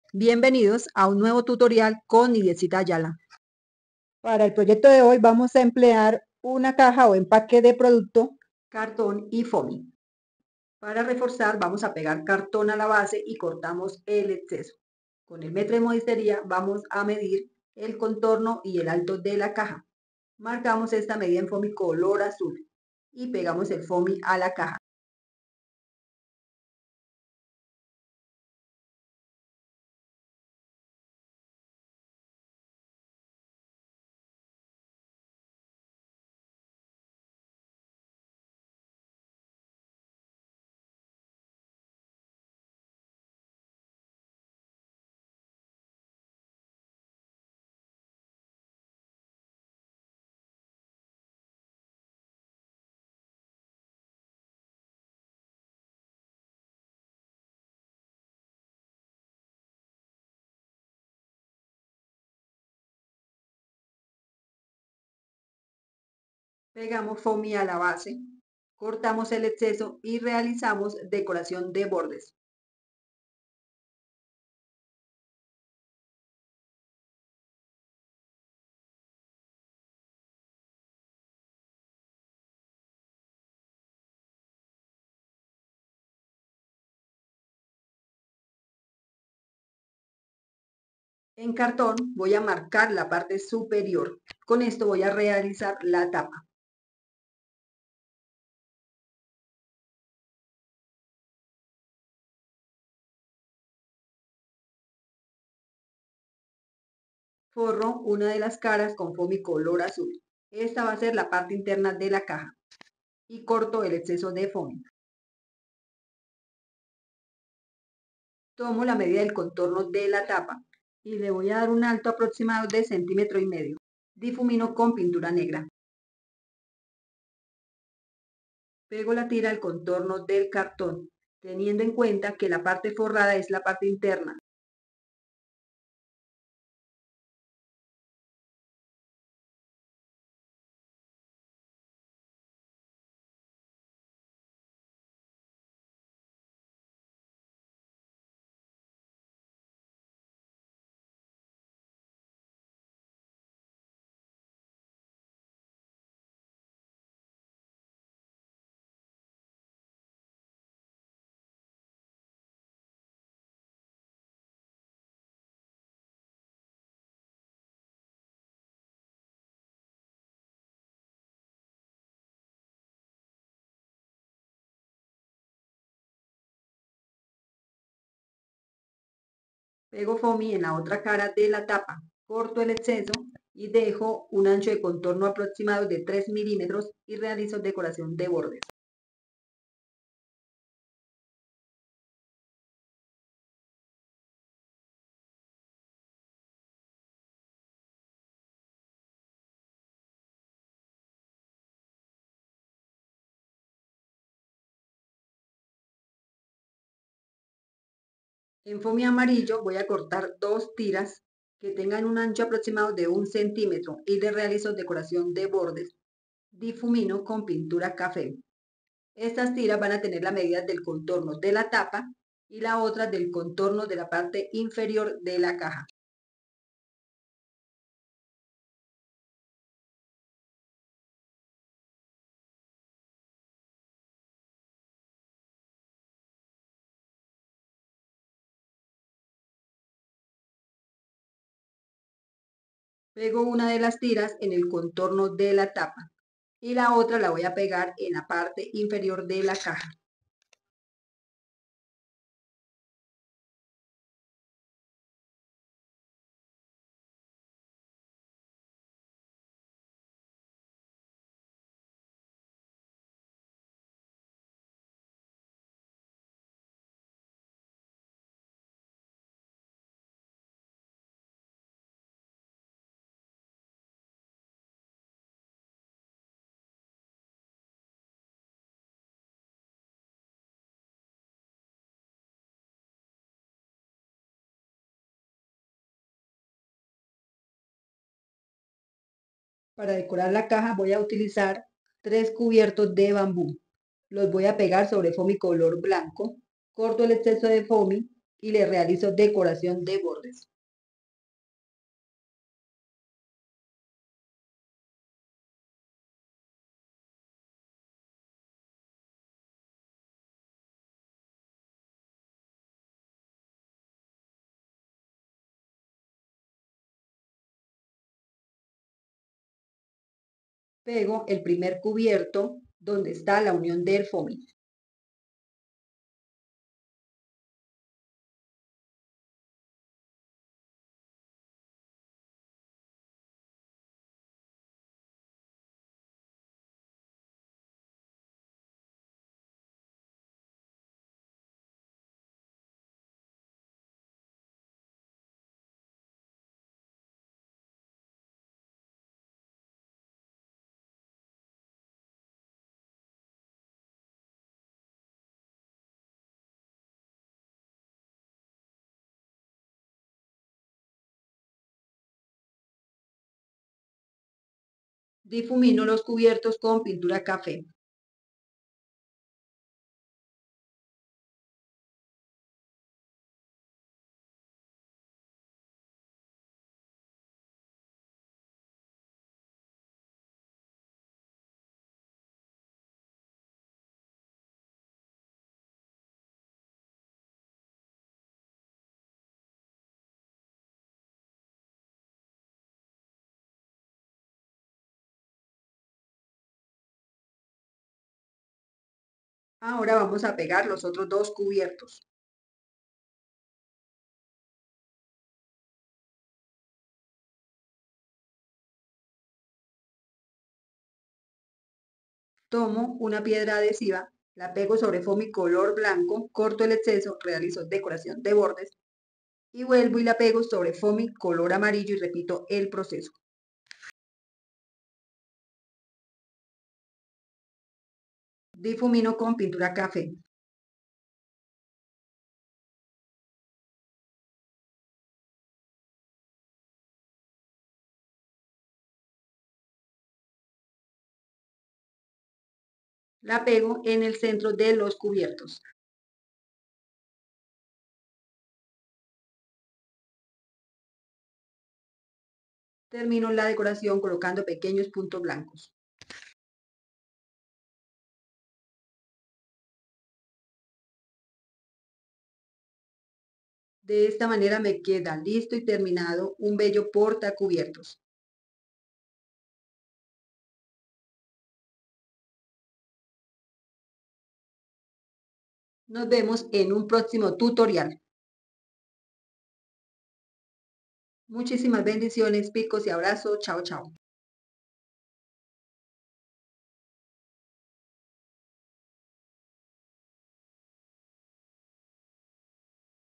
PORTACUBIERTOS CON CAJA DE LECHE Y FOMY
Recicla un empaque de leche y realiza un bonito y practico organizador de cubiertos